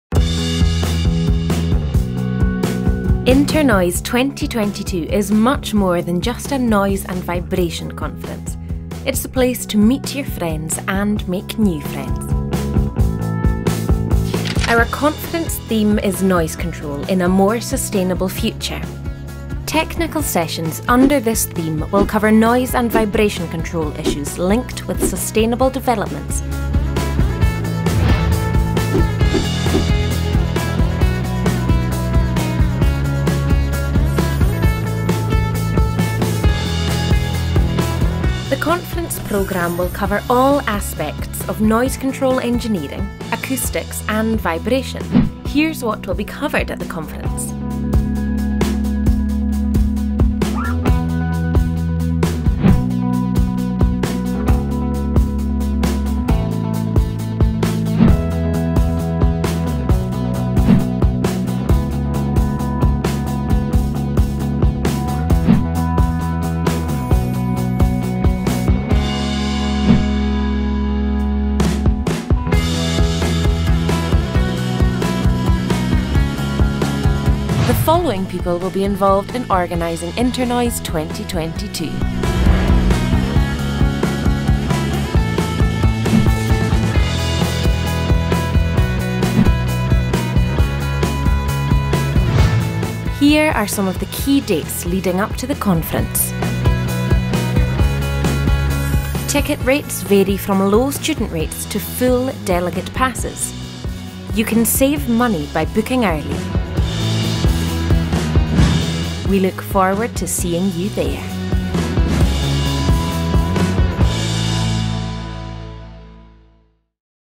inter noise 2022 video (2 mins )
Scotland calls!  The 51st International Congress and Exposition on Noise Control Engineering is to be held at the Scottish Event Campus (SEC) in Glasgow on the 21-24 August 2022. This is the major international conference on noise control engineering and attracts scientists, engineers and consultants from around the world.

The Congress theme is Noise Control in a more Sustainable Future. This theme is particularly timely and the congress venue is the same as that for the recent conference on climate change COP 26.

The Technical Program will have sessions in Main Topics ranging from Aircraft Noise to Underwater, Ship and Offshore Acoustics. Included is the Topic: Profession, Training and Outreach, which will be of special interest to young researchers, engineers and consultants who are at the early stage of their careers.

The Congress is organised by the Institute of Acoustics (IOA) and the United Kingdom Acoustics Network (UKAN) on behalf of the International Institute of Noise Control Engineering (I-INCE).